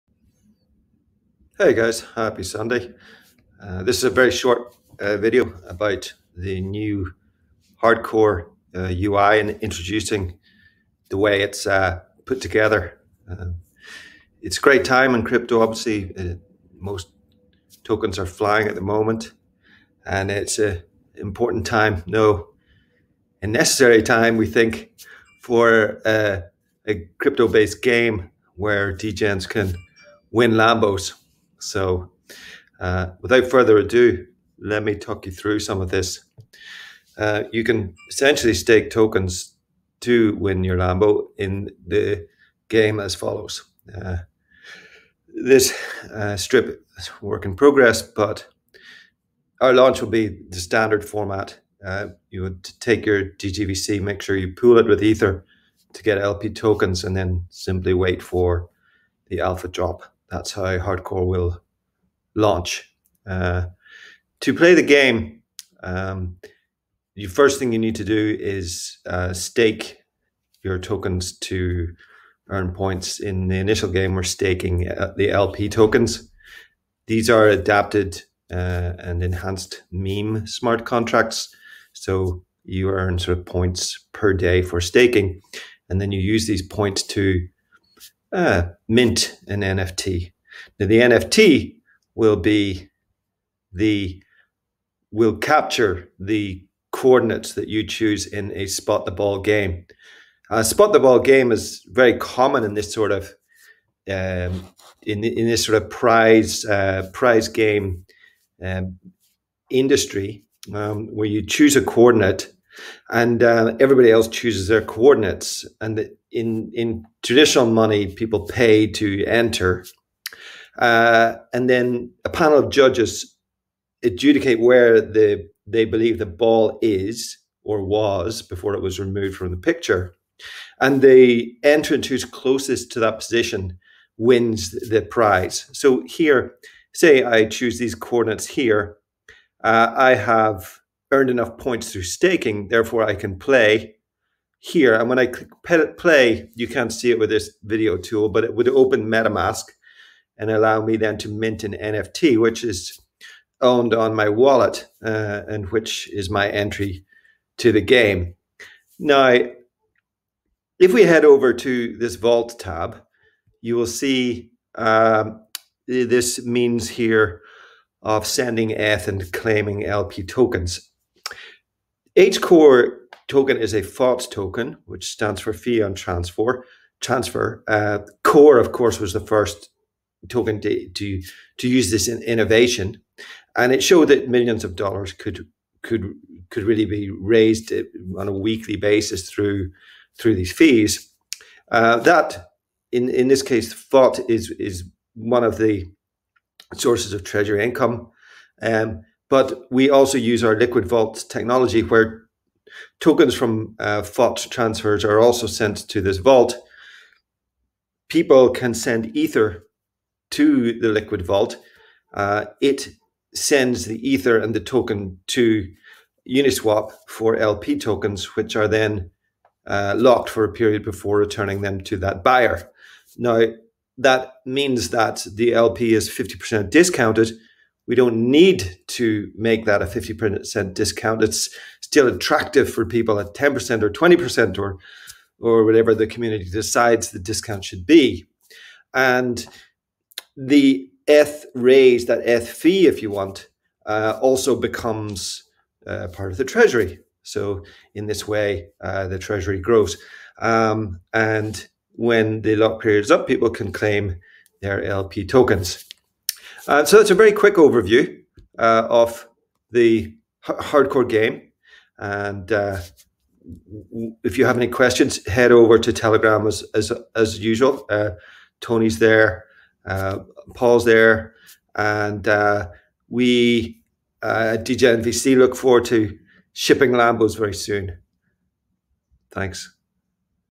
Introduction to HardCore's new UI.
In this video, Fraser walks us through the newly-designed UI for the HardCore project which we will be alphadropping to our degenhorde very soon! Check it out, and likes & comments always welcome!